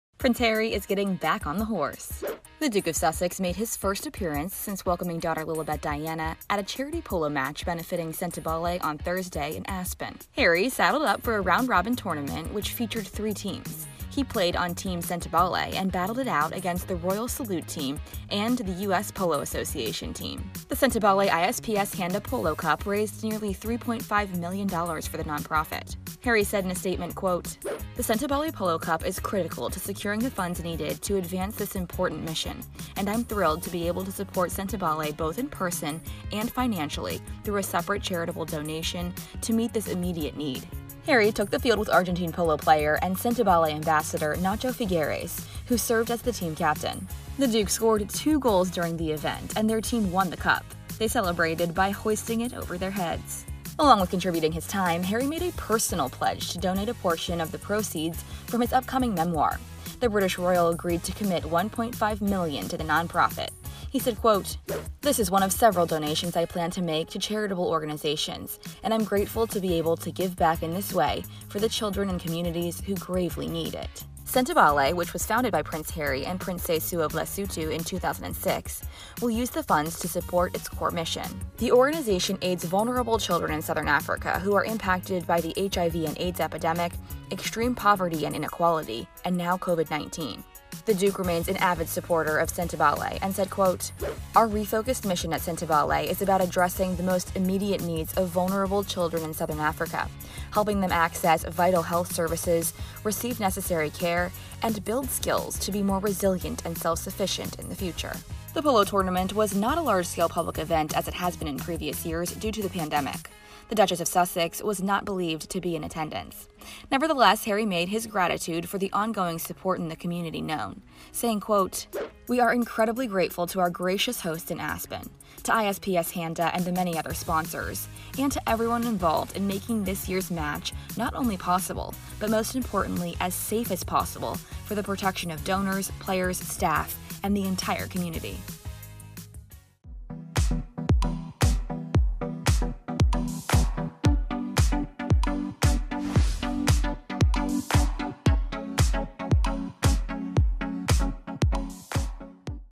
Prince Harry Joins Polo Cup In First Appearance Since Lilibet’s Birth
Prince Harry is getting back on the horse! The Duke of Sussex made his first appearance since welcoming daughter Lilibet Diana, at a Charity Polo match benefitting Sentebale on Thursday in Aspen. Harry saddled up for a round robin tournament which featured 3 teams. The Sentebale ISPS Handa Polo Cup raised nearly $3.5 million for the non-profit, which The Duke of Sussex co-founded in 2006. Harry agreed to personally commit $1.5 million, a portion of the proceeds from his upcoming memoir.